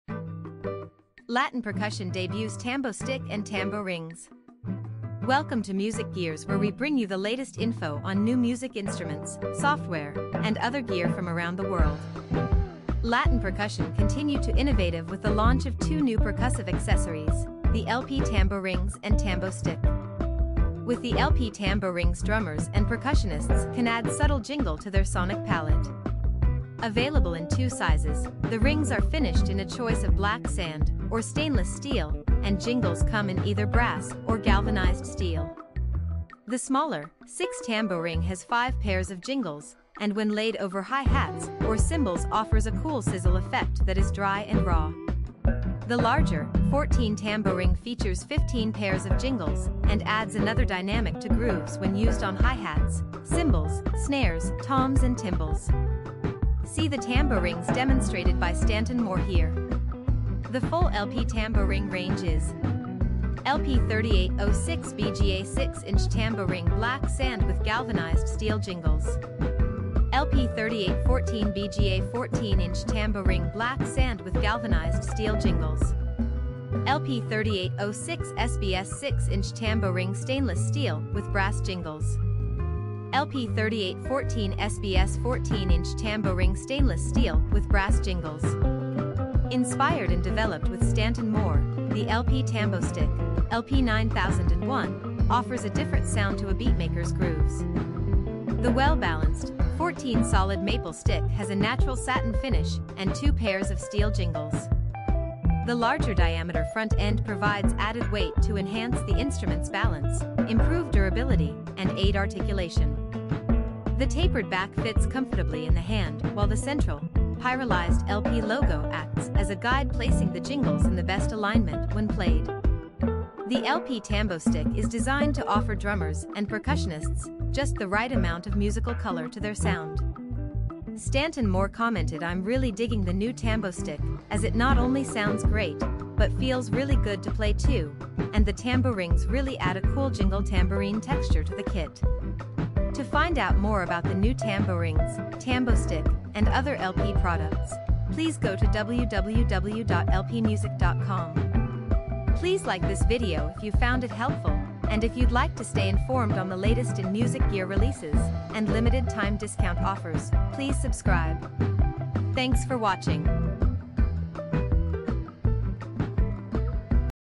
Latin Percussion Debuts Tambo-Stick & Tambo-Rings
Latin Percussion continue to innovative with the launch of two new percussive accessories: the LP Tambo-Rings and Tambo-Stick.

With the LP Tambo-Rings drummers and percussionists can add subtle jingle to their sonic palette. Available in two sizes, the rings are finished in a choice of black sand or stainless steel and jingles come in either brass or galvanized steel. The smaller, 6" Tambo-Ring has five pairs of jingles and when laid over hi hats or cymbals offers a cool sizzle effect that is dry and raw. The larger, 14” Tambo-Ring features 15 pairs of jingles and adds another dynamic to grooves when used on hi-hats, cymbals, snares, toms and timbales. See the Tambo-Rings demonstrated by Stanton Moore here. The full LP Tambo-Ring range is:

LP3806BGA 6-Inch Tambo-Ring - Black Sand with Galvanized Steel Jingles
LP3814BGA 14-Inch Tambo-Ring - Black Sand with Galvanized Steel Jingles
LP3806SBS 6-Inch Tambo-Ring - Stainless Steel with Brass Jingles
LP3814SBS 14-Inch Tambo-Ring - Stainless Steel with Brass Jingles

Inspired and developed with Stanton Moore, the LP Tambo-Stick (LP9001) offers a different sound to a beatmaker’s grooves. The well balanced, 14" solid maple stick has a natural satin finish and two pairs of steel jingles. The larger diameter front end provides added weight to enhance the instrument’s balance, improve durability and aide articulation. The tapered back fits comfortably in the hand while the central, pyrolyzed LP logo acts as a guide placing the jingles in the best alignment when played. The LP Tambo-Stick is designed to offer drummers and percussionists just the right amount of musical color to their sound.

Stanton Moore commented “I’m really digging the new Tambo-Stick as it not only sounds great but feels really good to play too, and the Tambo-Rings really add a cool jingle tambourine texture to the kit.”

To find out more about the new Tambo-Rings, Tambo-Stick and other LP products, please go